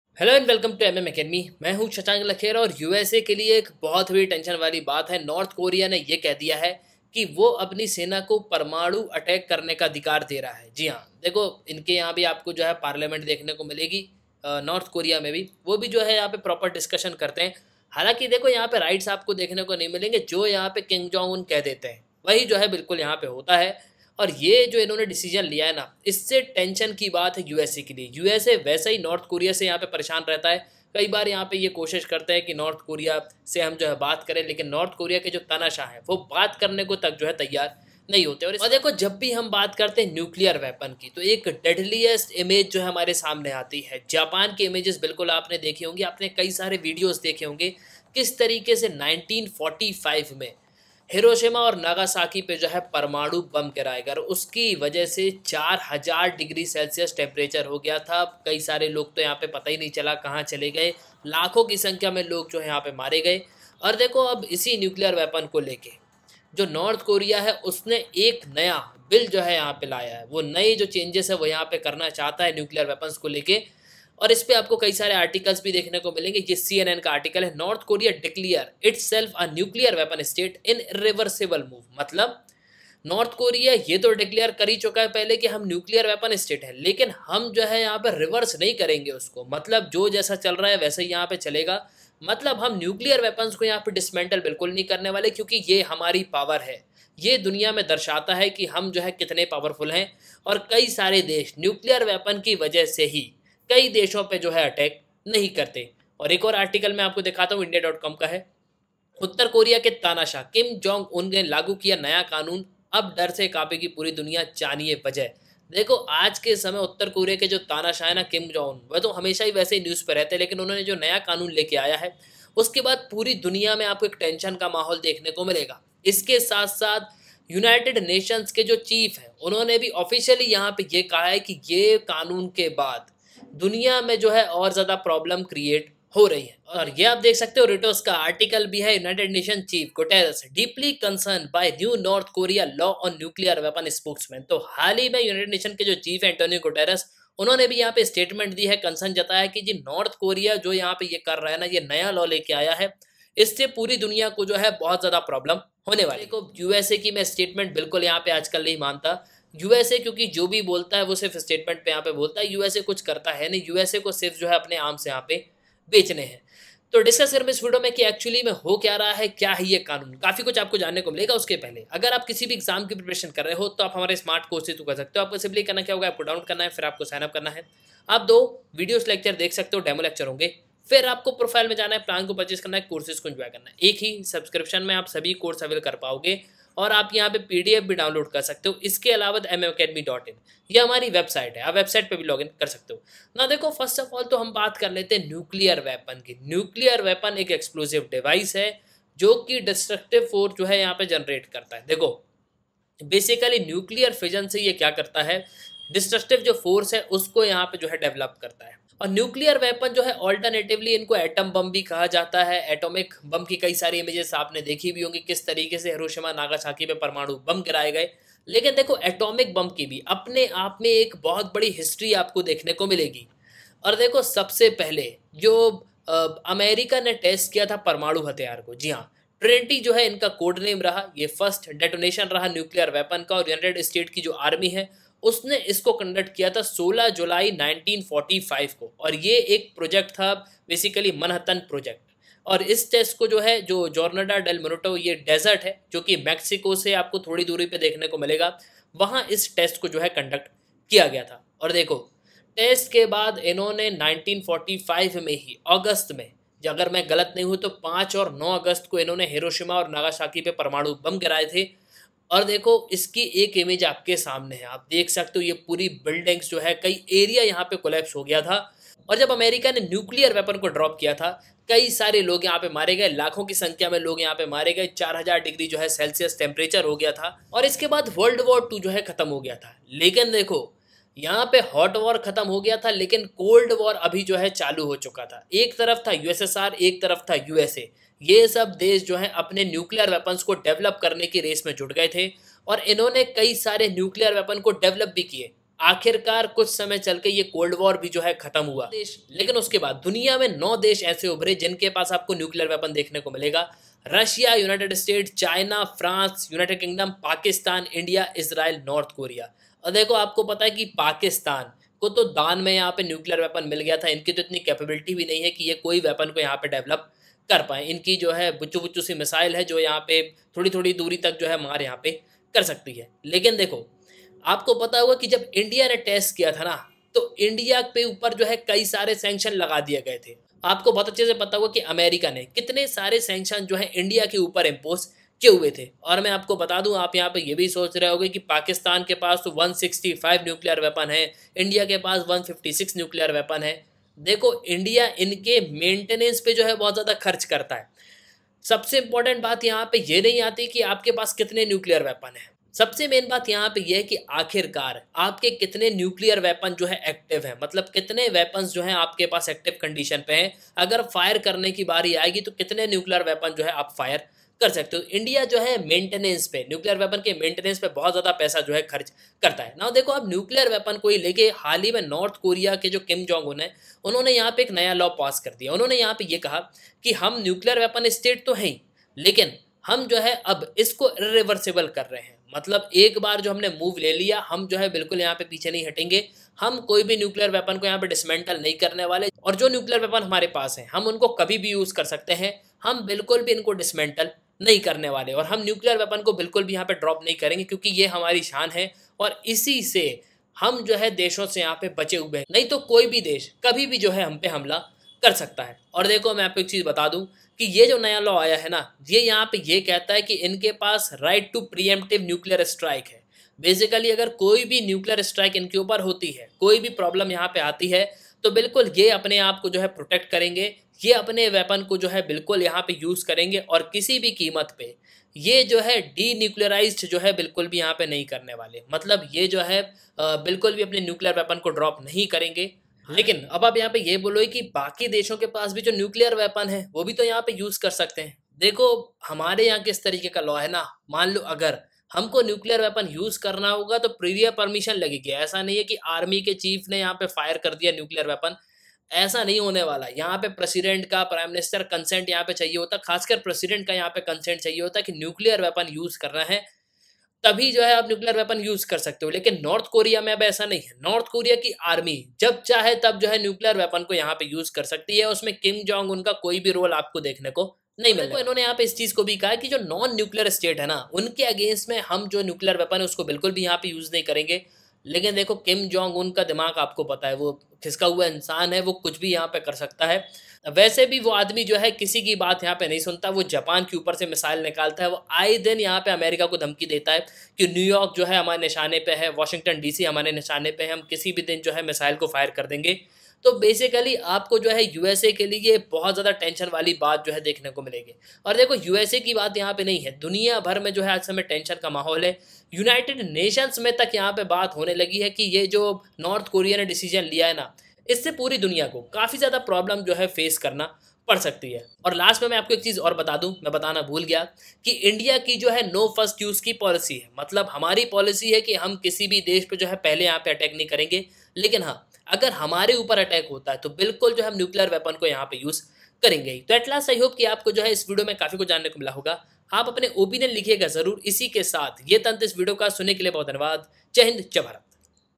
Big Tension For USA l North Korea ने सेना को दिया परमाणु अटैक करने का अधिकार
Get an Awesome courses for UPSC and State PCS on MM Academy App

For Demo lectures-